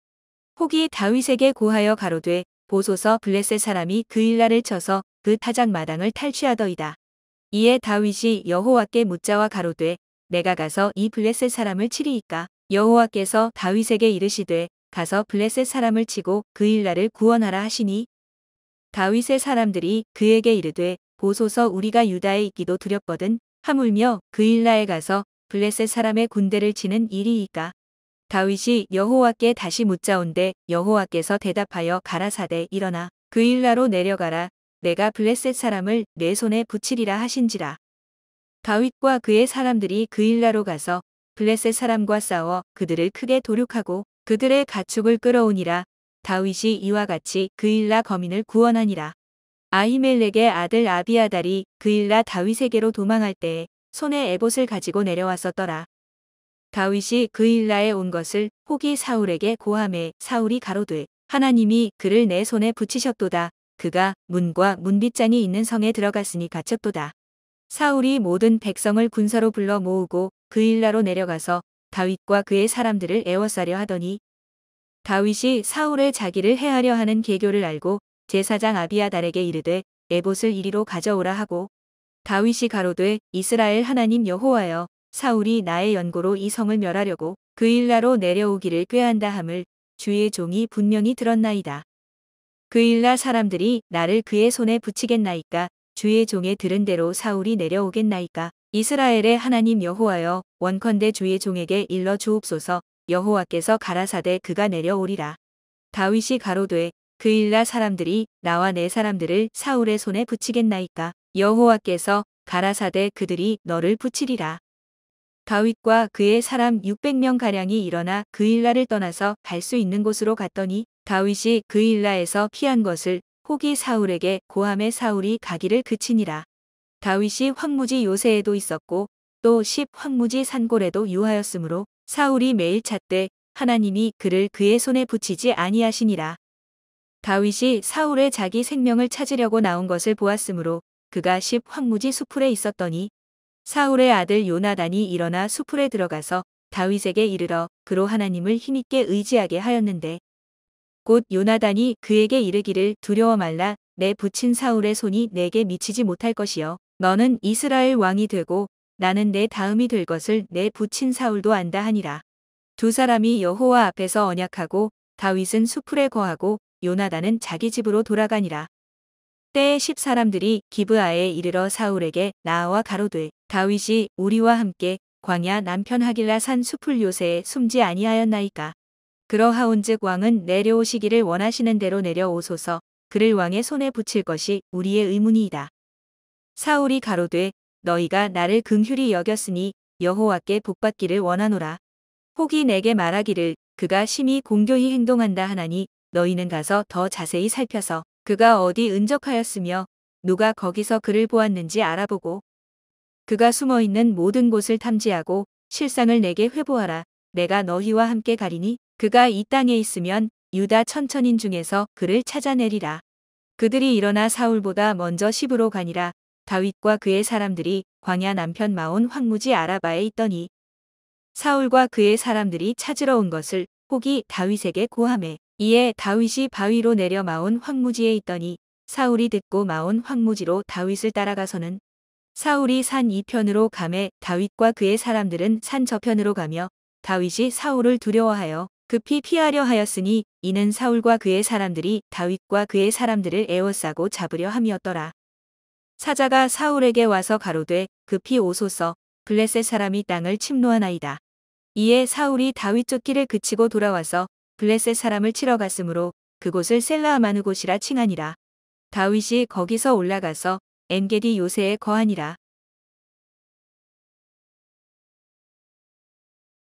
💚 사무엘상 23장 낭독 | 혹이 다윗에게 고하여 가로되 보소서 블레셋 사람이 그일라를 쳐서 그 타작 마당을 탈취하더이다 | 네이버클로바더빙
사무엘상 23장 낭독 입니다.

1절 혹이 다윗에게 고하여 가로되 보소서 블레셋 사람이 그일라를 쳐서 그 타작 마당을 탈취하더이다
2절 이에 다윗이 여호와께 묻자와 가로되 내가 가서 이 블레셋 사람을 치리이까 여호와께서 다윗에게 이르시되 가서 블레셋 사람을 치고 그일라를 구원하라 하시니
3절 다윗의 사람들이 그에게 이르되 보소서 우리가 유다에 있기도 두렵거든 하물며 그일라에 가서 블레셋 사람의 군대를 치는 일이리이까
4절 다윗이 여호와께 다시 묻자온대 여호와께서 대답하여 가라사대 일어나 그일라로 내려가라 내가 블레셋 사람을 네 손에 붙이리라 하신지라
5절 다윗과 그의 사람들이 그일라로 가서 블레셋 사람과 싸워 그들을 크게 도륙하고 그들의 가축을 끌어오니라 다윗이 이와 같이 그일라 거민을 구원하니라
6절 아히멜렉의 아들 아비아달이 그일라 다윗에게로 도망할 때에 손에 에봇을 가지고 내려왔었더라
7절 다윗이 그일라에 온 것을 혹이 사울에게 고하매 사울이 가로되 하나님이 그를 내 손에 붙이셨도다 그가 문과 문빗장이 있는 성에 들어갔으니 갇혔도다
8절 사울이 모든 백성을 군사로 불러 모으고 그일라로 내려가서 다윗과 그의 사람들을 에워싸려 하더니
9절 다윗이 사울의 자기를 해하려 하는 계교를 알고 제사장 아비아달에게 이르되 에봇을 이리로 가져오라 하고
10절 다윗이 가로되 이스라엘 하나님 여호와여 사울이 나의 연고로 이 성을 멸하려고 그일라로 내려오기를 꾀한다 함을 주의 종이 분명히 들었나이다
11절 그일라 사람들이 나를 그의 손에 붙이겠나이까 주의 종의 들은 대로 사울이 내려 오겠나이까 이스라엘의 하나님 여호와여 원컨대 주의 종에게 일러 주옵소서 여호와께서 가라사대 그가 내려 오리라
12절 다윗이 가로되 그일라 사람들이 나와 내 사람들을 사울의 손에 붙이겠나이까 여호와께서 가라사대 그들이 너를 붙이리라
13절 다윗과 그의 사람 육백 명 가량이 일어나 그일라를 떠나서 갈 수 있는 곳으로 갔더니 다윗이 그일라에서 피한 것을 혹이 사울에게 고하매 사울이 가기를 그치니라
14절 다윗이 황무지 요새에도 있었고 또 십 황무지 산골에도 유하였으므로 사울이 매일 찾되 하나님이 그를 그의 손에 붙이지 아니하시니라
15절 다윗이 사울의 자기 생명을 찾으려고 나온 것을 보았으므로 그가 십 황무지 수풀에 있었더니
16절 사울의 아들 요나단이 일어나 수풀에 들어가서 다윗에게 이르러 그로 하나님을 힘있게 의지하게 하였는데
17절 곧 요나단이 그에게 이르기를 두려워 말라 내 부친 사울의 손이 네게 미치지 못할 것이요 너는 이스라엘 왕이 되고 나는 네 다음이 될 것을 내 부친 사울도 안다 하니라
18절 두 사람이 여호와 앞에서 언약하고 다윗은 수풀에 거하고 요나단은 자기 집으로 돌아가니라
19절 때에 십 사람들이 기브아에 이르러 사울에게 나아와 가로되 다윗이 우리와 함께 광야 남편 하길라 산 수풀 요새에 숨지 아니하였나이까
20절 그러하온즉 왕은 내려오시기를 원하시는 대로 내려 오소서 그를 왕의 손에 붙일 것이 우리의 의무니이다
21절 사울이 가로되 너희가 나를 긍휼히 여겼으니 여호와께 복받기를 원하노라
22절 혹이 내게 말하기를 그가 심히 공교히 행동한다 하나니 너희는 가서 더 자세히 살펴서 그가 어디 은적하였으며 누가 거기서 그를 보았는지 알아보고
23절 그가 숨어 있는 모든 곳을 탐지하고 실상을 내게 회보하라 내가 너희와 함께 가리니 그가 이 땅에 있으면 유다 천천인 중에서 그를 찾아내리라
24절 그들이 일어나 사울보다 먼저 십으로 가니라 다윗과 그의 사람들이 광야 남편 마온 황무지 아라바에 있더니
25절 사울과 그의 사람들이 찾으러 온 것을 혹이 다윗에게 고하매 이에 다윗이 바위로 내려 마온 황무지에 있더니 사울이 듣고 마온 황무지로 다윗을 따라가서는
26절 사울이 산 이편으로 가매 다윗과 그의 사람들은 산 저편으로 가며 다윗이 사울을 두려워하여 급히 피하려 하였으니 이는 사울과 그의 사람들이 다윗과 그의 사람들을 에워싸고 잡으려 함이었더라
27절 사자가 사울에게 와서 가로되 급히 오소서 블레셋 사람이 땅을 침로하나이다
28절 이에 사울이 다윗 쫓기를 그치고 돌아와서 블레셋 사람을 치러 갔으므로 그곳을 셀라하마느곳이라 칭하니라
29절 다윗이 거기서 올라가서 엔게디 요새에 거하니라

사무엘상
성경낭독
성경읽기
성경암송
성경듣기
오디오성경
Korean The Holy Bible Samuel